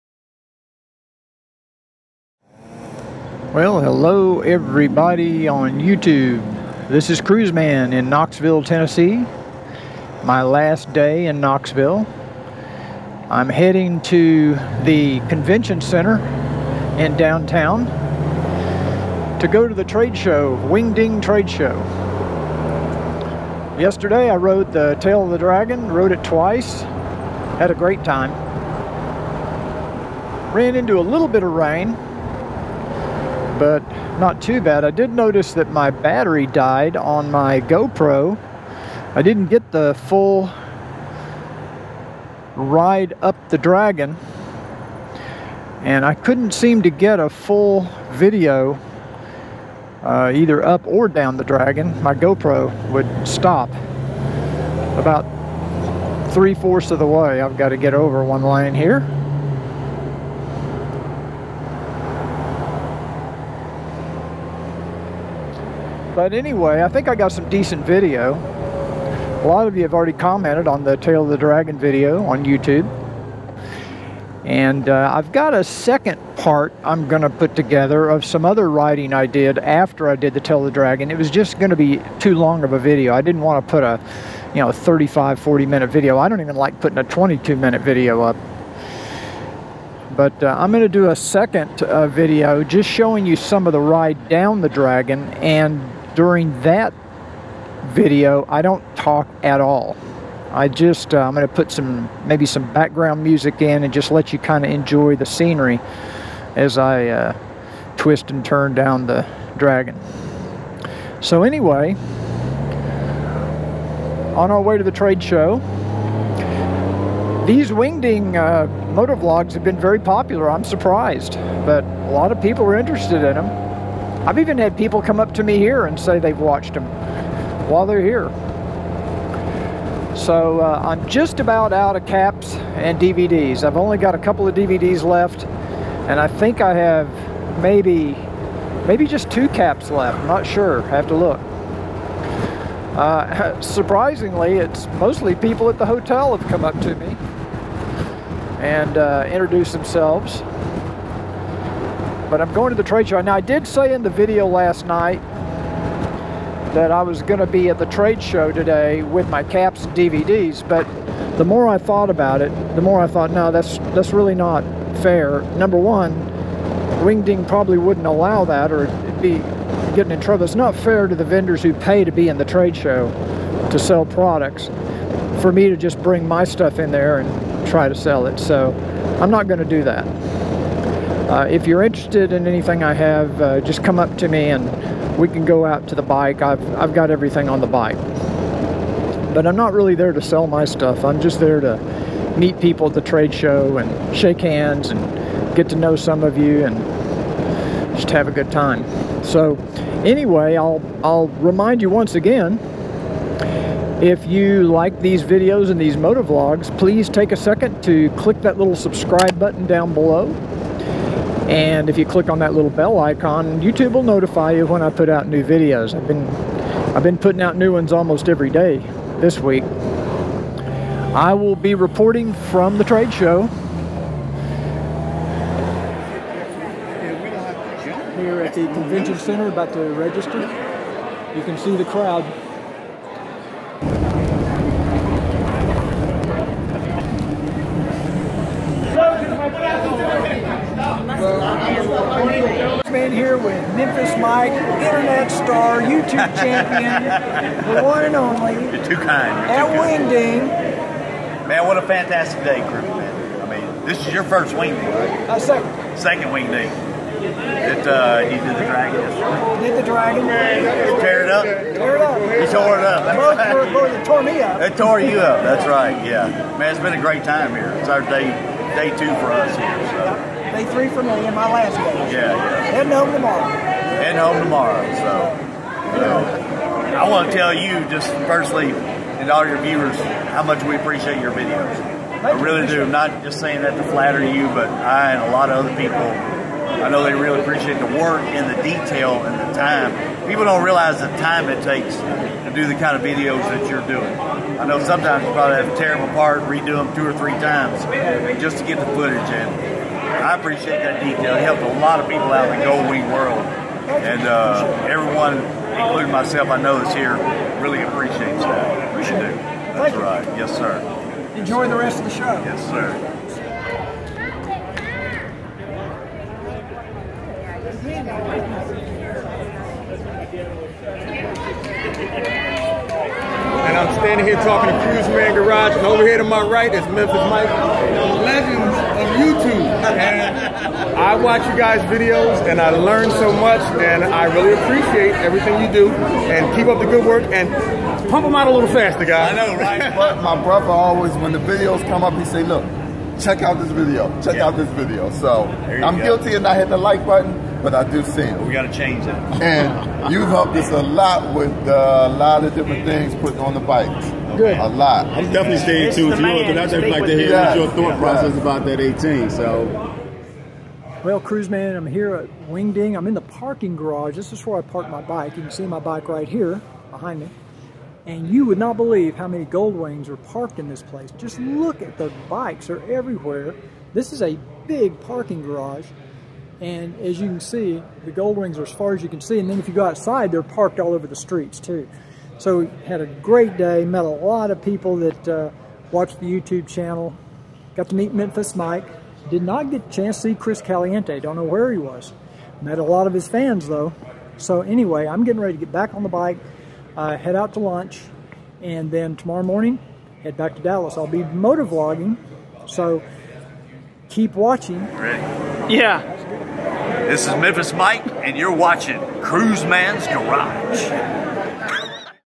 [Ep 20] Wing Ding Trade Show - 2018 Honda Goldwing
Cruiseman visits the trade show and meets up with lots of YouTube followers and other YouTube personalities.

Background music under license from SmartSound®

You can find Cruiseman on TheGLForum.com